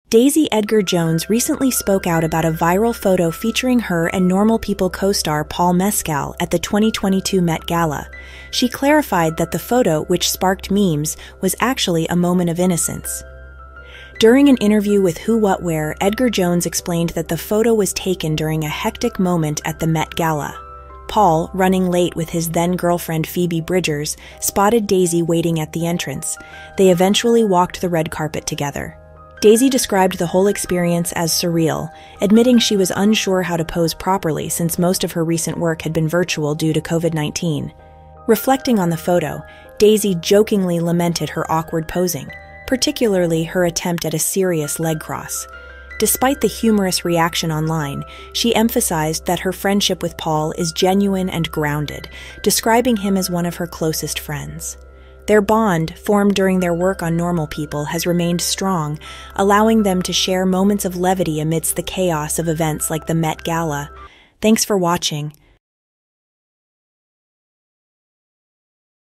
Daisy Edgar Jones Decodes Viral Meme with Paul Mescal & Phoebe Bridgers at 2022 Met Gala
Daisy Edgar Jones recently addressed the viral meme featuring her and Paul Mescal at the 2022 Met Gala. She shared the story behind the photo, which captured a moment of innocence amid the event's chaos. Daisy described how their strong friendship, formed during their work on Normal People, added humor to the experience. Watch to find out what really happened at the Met Gala!